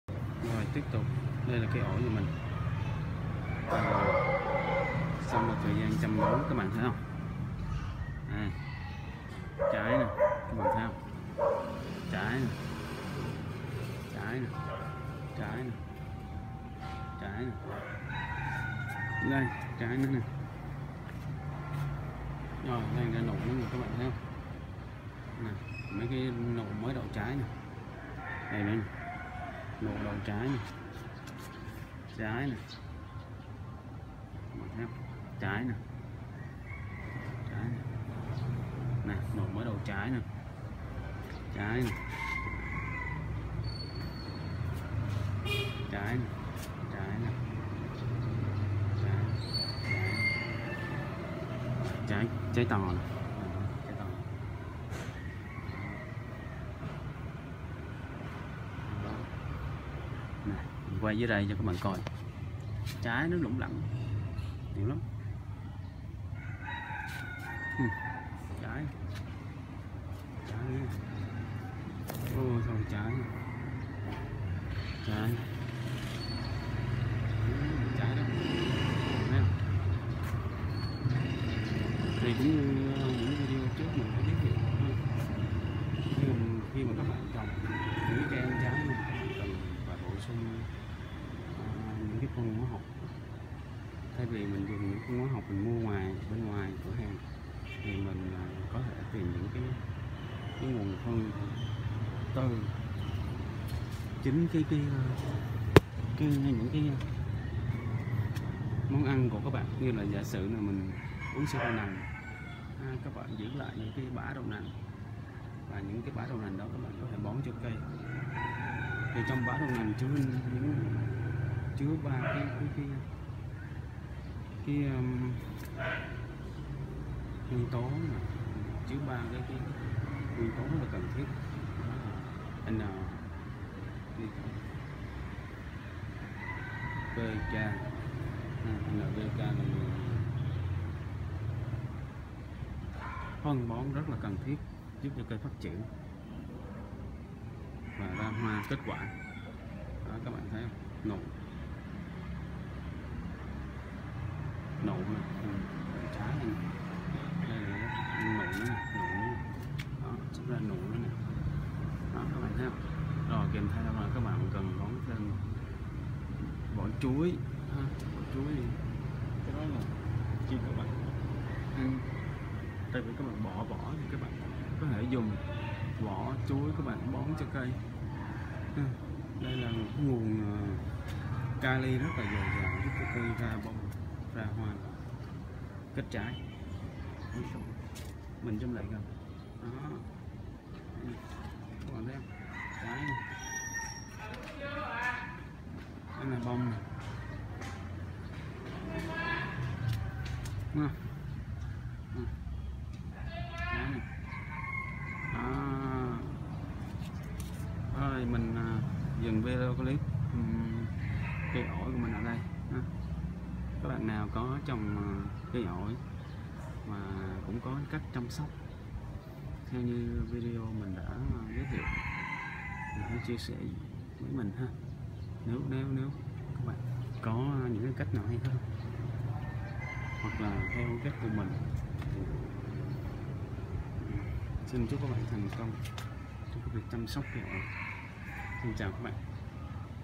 Chăm sóc cây ổi mau ra hoa đậu trái - thành quả
Sau một thời gian chăm sóc, cây ổi nhà mình ra rất nhiều trái và tiếp tục ra nụ không ngừng ...